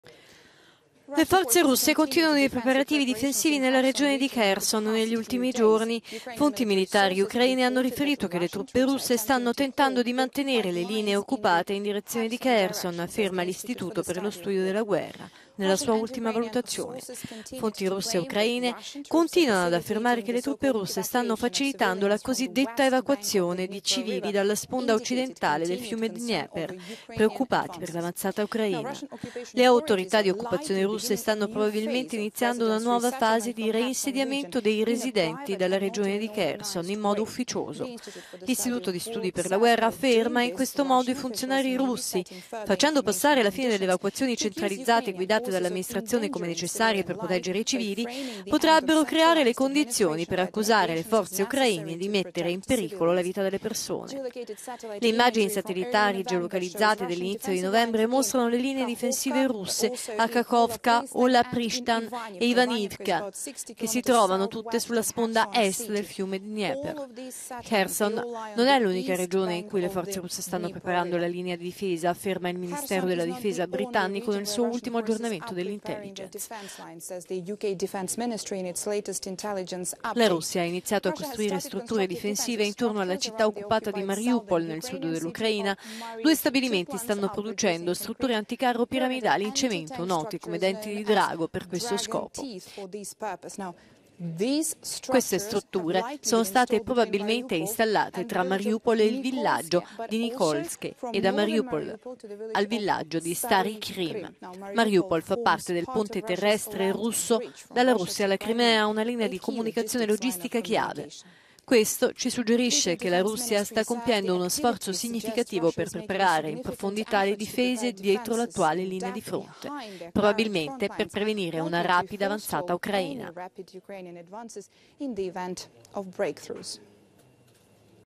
Ucraina: i russi schierano i 'denti di drago' sulla linea difensiva
Mosca teme la controffensiva di Kiev; continua l'evacuazione dei civili a Kherson